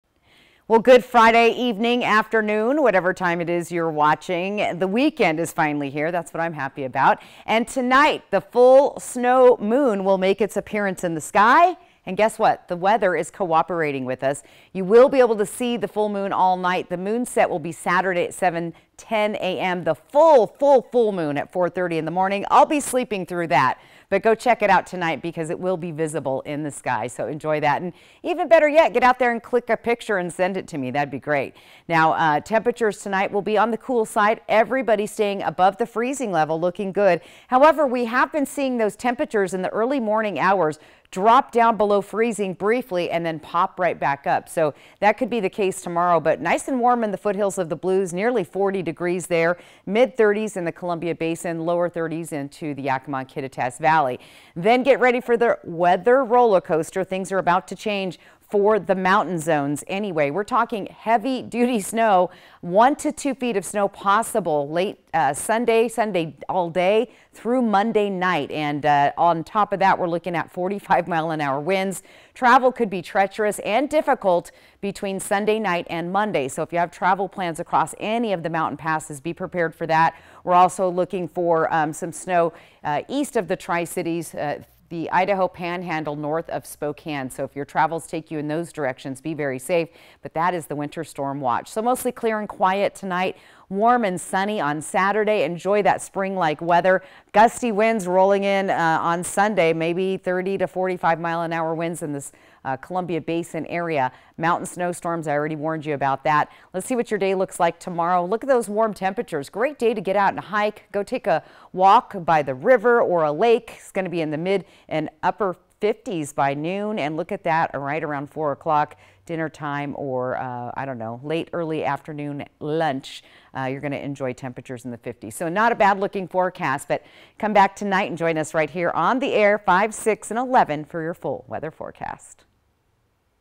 First Alert Weather Friday Evening
Rollercoaster Weather Ahead: from Clear Skies to Snowstorm in the Pacific Northwest